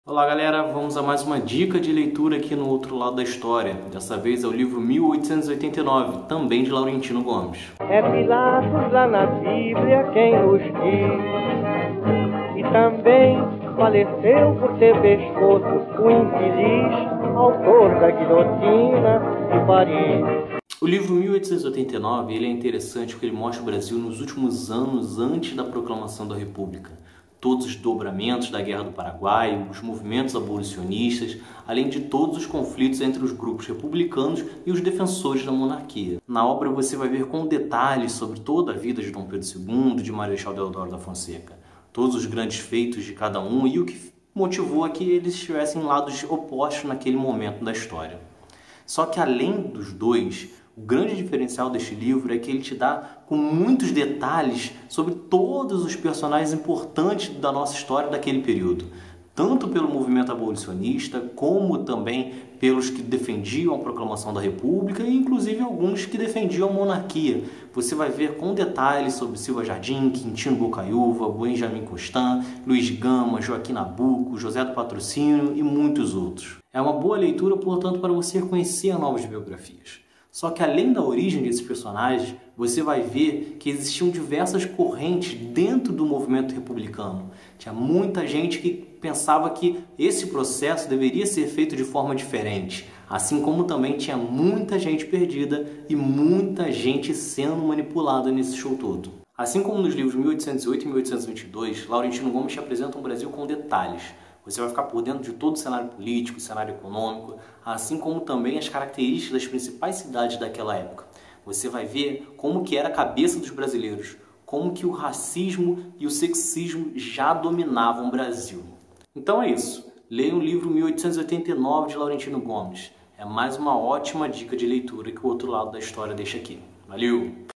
Livro 1889 - Entenda como estava o Brasil antes da Proclamação da República
Vale a leitura: Livro 1889 - Laurentino Gomes

Confira um resumo do que você vai encontrar neste excelente livro de Laurentino Gomes, que encerra a trilogia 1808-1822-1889 e mostra os últimos anos antes da Proclamação da República do Brasil.